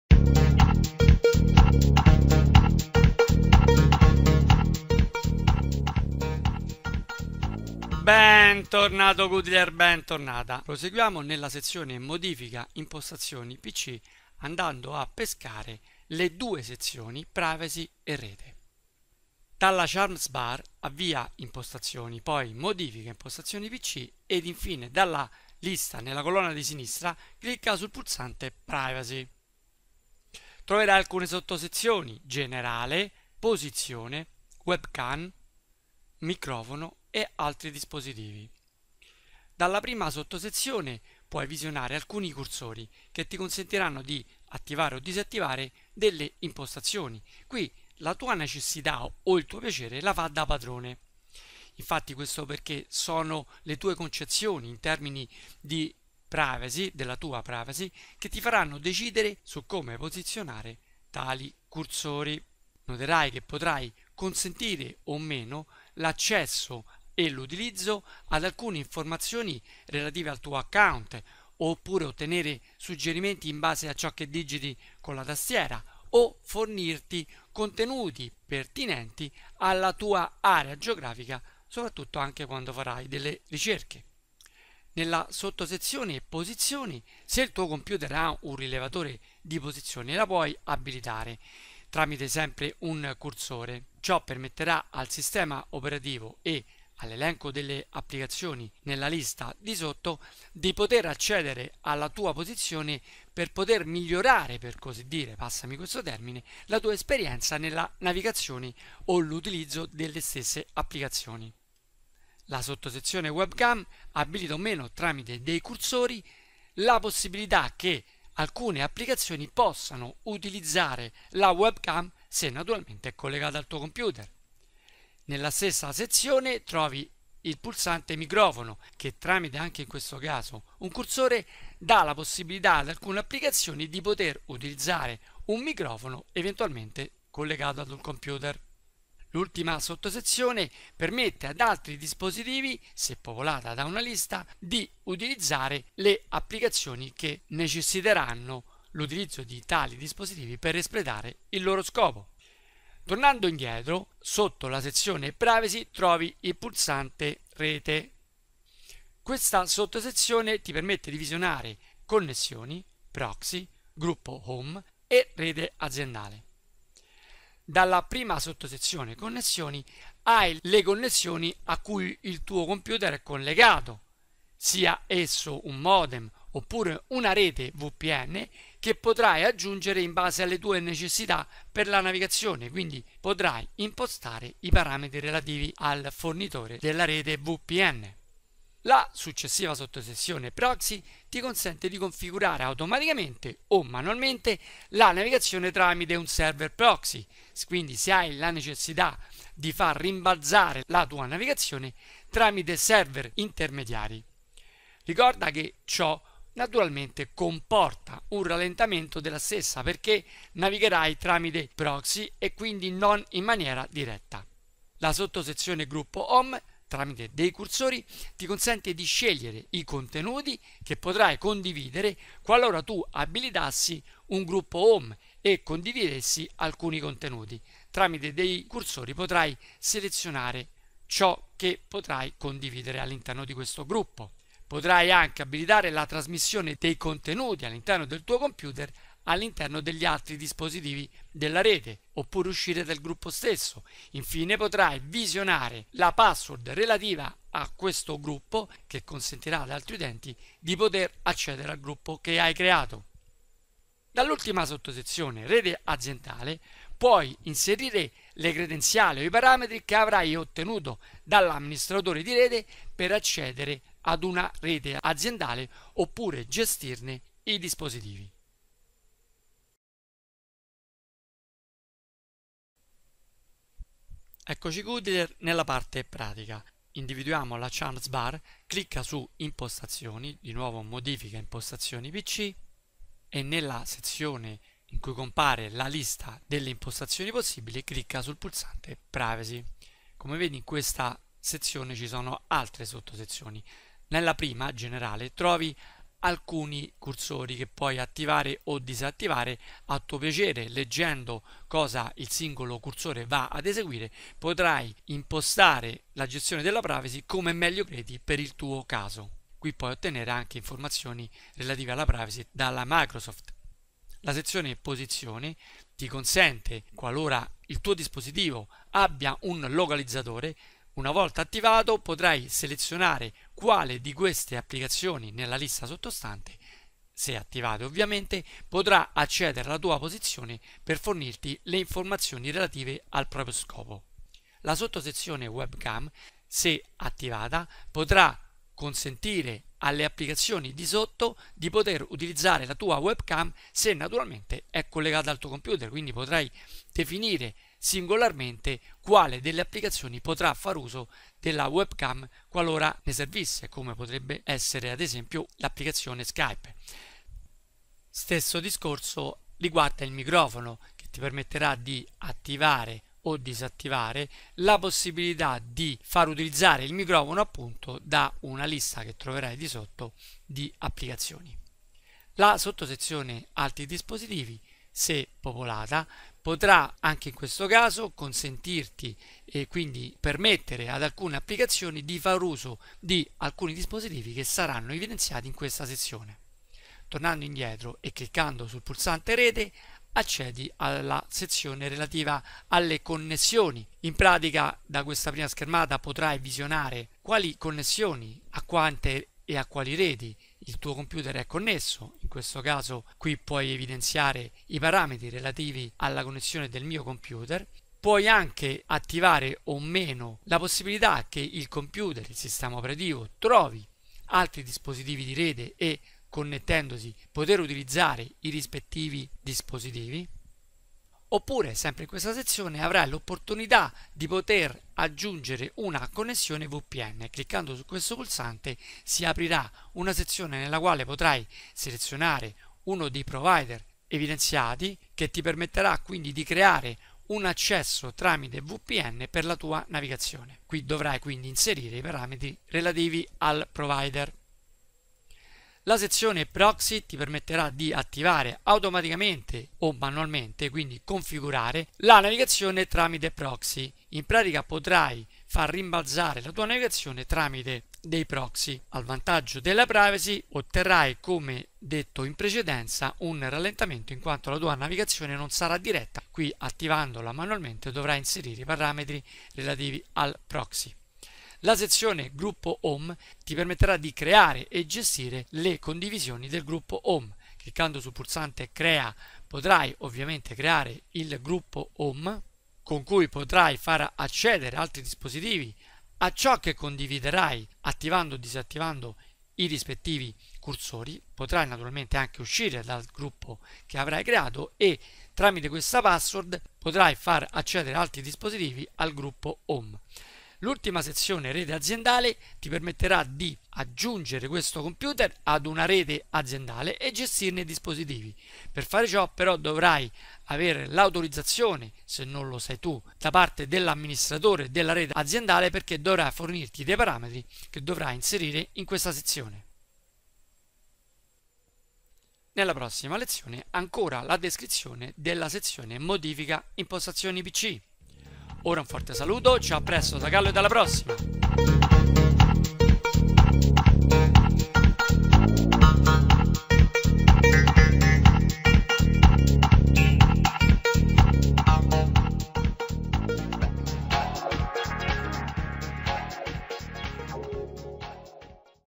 Guida al Computer - Lezione 150 - Windows 8.1 Update – Modifica Impostazioni PC Parte 4
Quarta parte della sezione modifica impostazioni PC in Windows 8.1 Update. Si tratta la privacy impostando i consensi per la sua gestione ed i parametri e gestione delle tue reti.